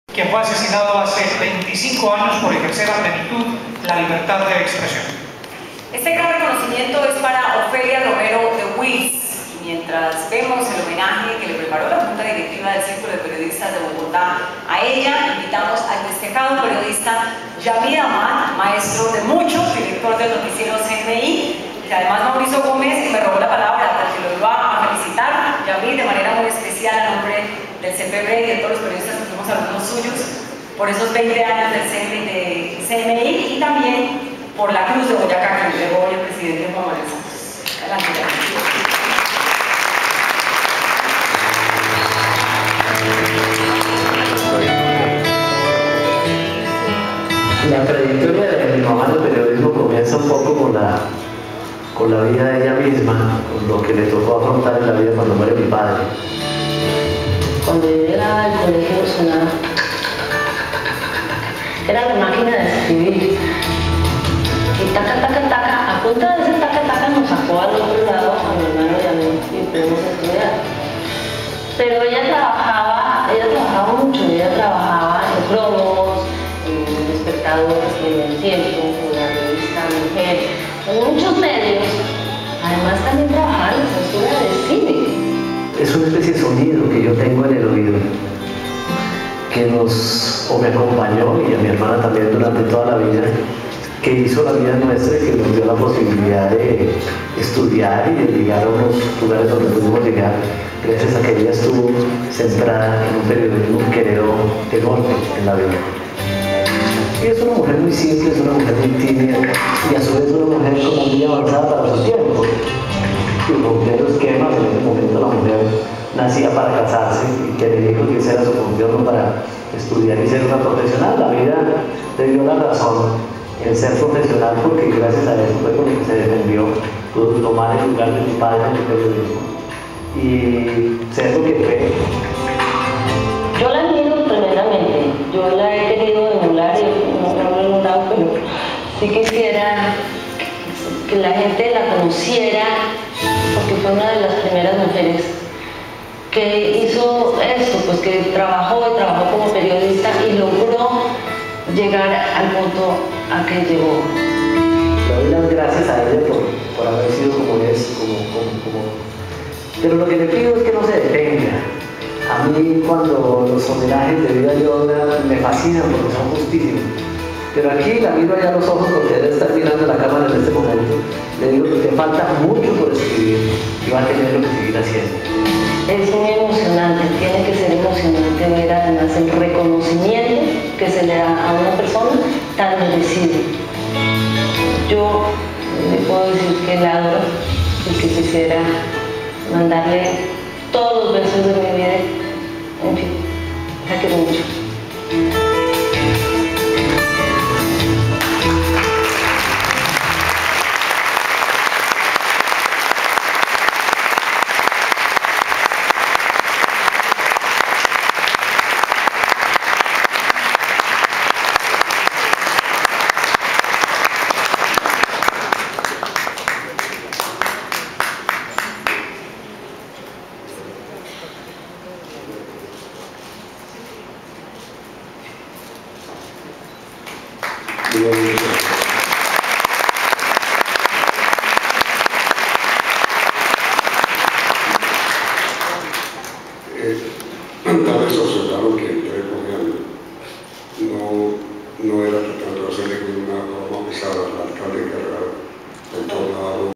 Ofelia Romero de Wills - Premio de Periodismo CPB
Ofelia Romero de Wills, una de las primeras mujeres que trabajó para los medios de comunicación en Colombia, brilló en diarios como La República, El Tiempo y El Espectador. A ella se le rindió tributo con el premio al Mérito Periodístico durante la ceremonia de los Premios de Periodismo CPB. Merecido reconocimiento.